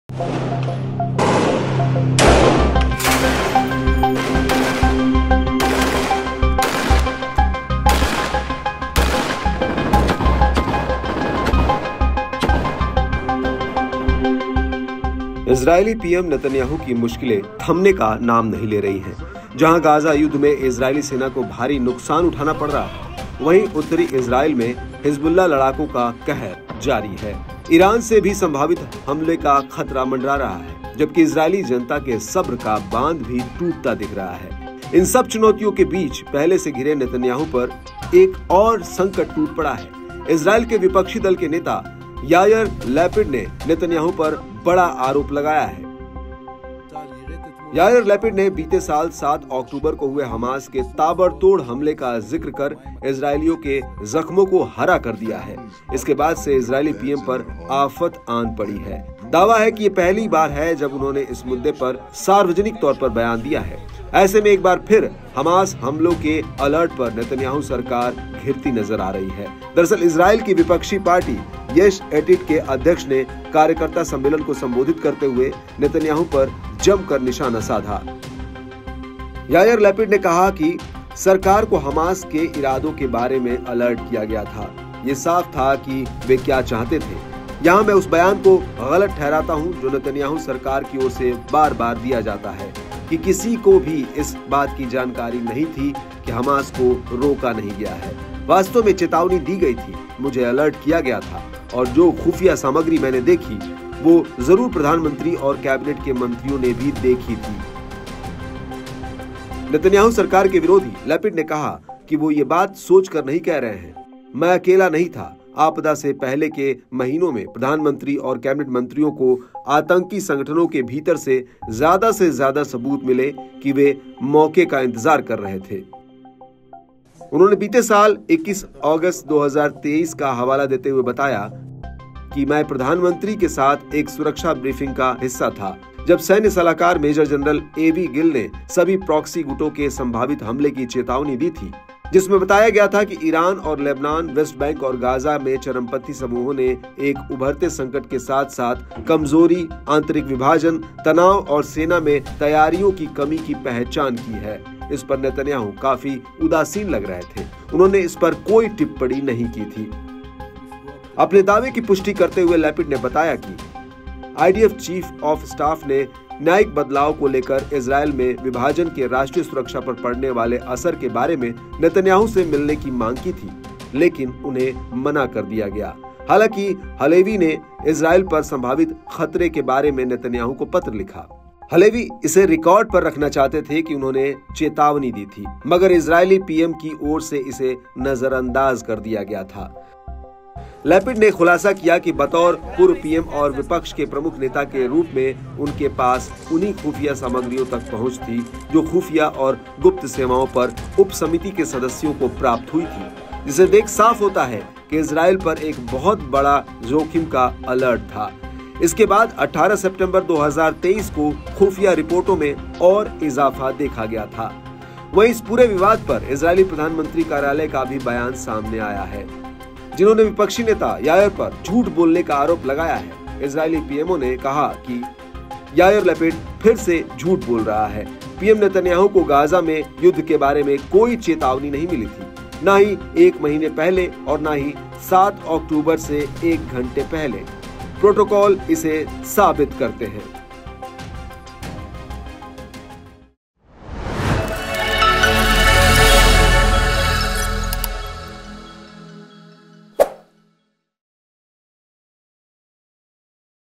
Israel Hamas War : North Israel में हिजबुल्लाह लड़ाकों का कहर जारी, दिलदहला देने वाली तस्वीरें! N18G
North Israel में हिजबुल्लाह लड़ाकों का कहर जारी, दिलदहला देने वाली तस्वीरें!